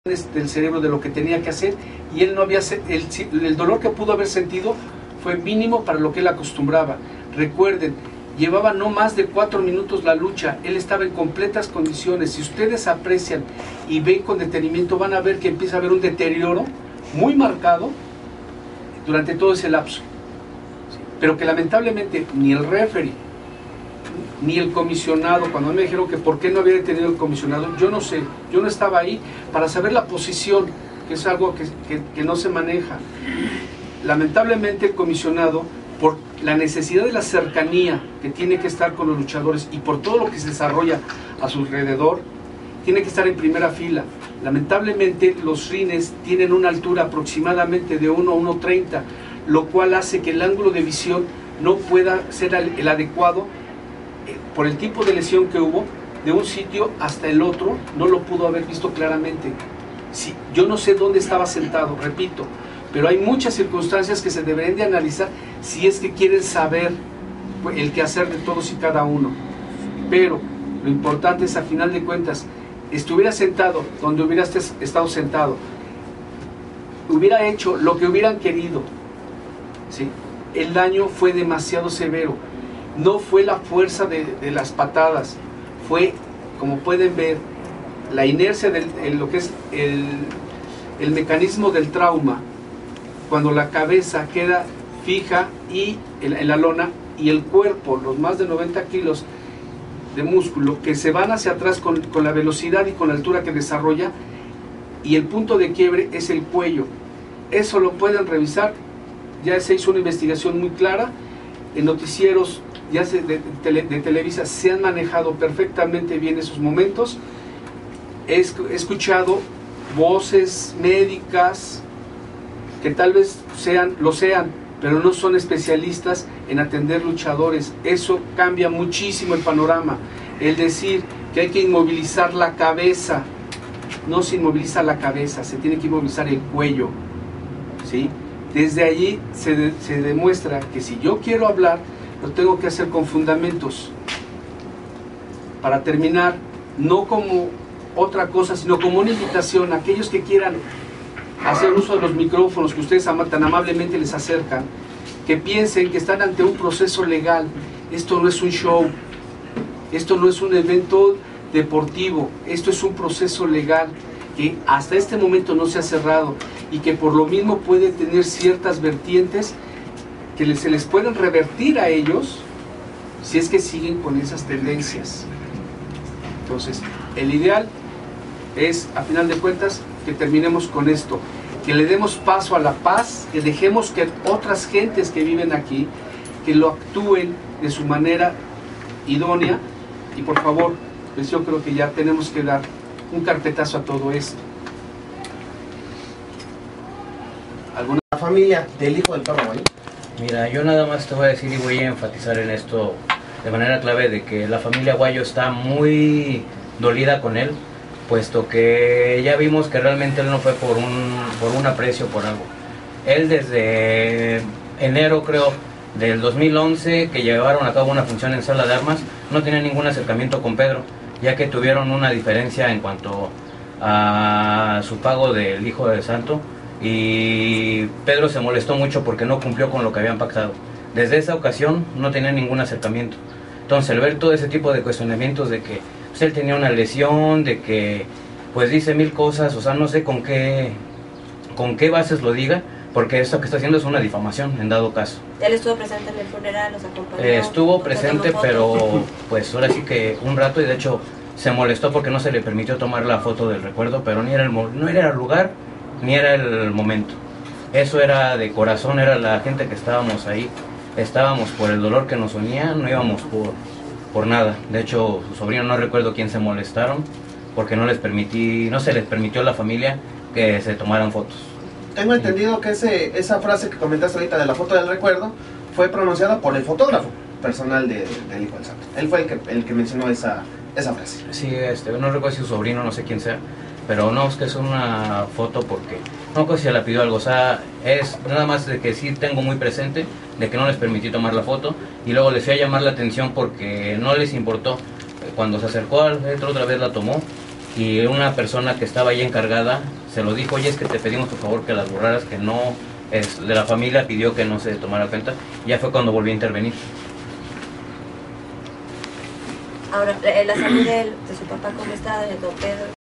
PRODUCCIONES PERROS DEL MAL PONEN UN ALTO A ESPECULACIONES PARTE 2
El Dr. Mario García Orozco y Khan, a nombre de la familia del Hijo de Perro Aguayo piden poner fin a las especulaciones.

Para dar fin a una serie de rumores y especulaciones, se llevó a cabo una conferencia de prensa en las instalaciones de la Comisión de Lucha Libre del Gobierno del Distrito Federal, en la cual tomaron parte el Dr. Mario García Orozco, médico de cabecera del Hijo de Perro Aguayo y amigo de la familia desde hace muchos años, Khan, familiar directo, managger y mano derecha de Pedro y El Fantasma, presidente de la Comisión del D. F.